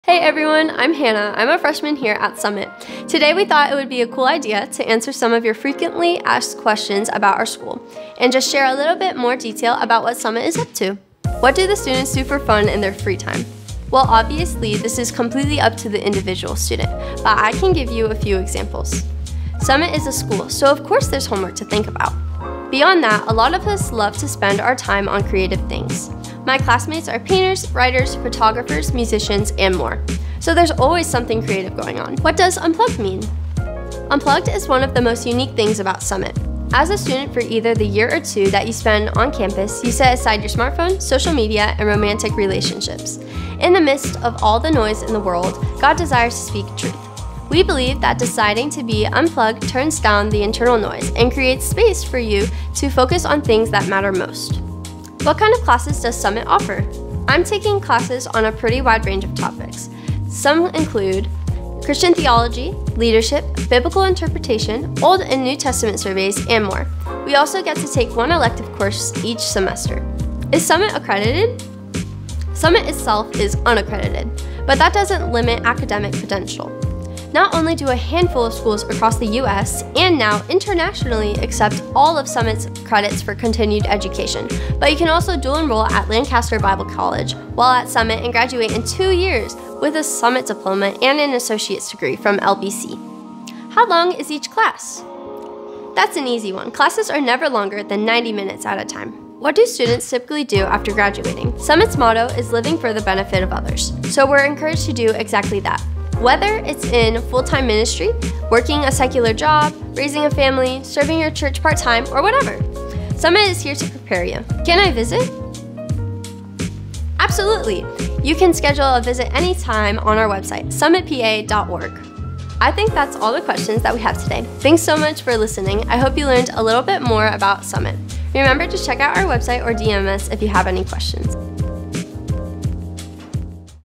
Answering Your Frequently Asked Questions
Summit International School of Ministry

In this video Summit Freshman, Hannah, answers some of your frequently asked questions about the Bible School of Times Square Church: Summit International School of Ministry.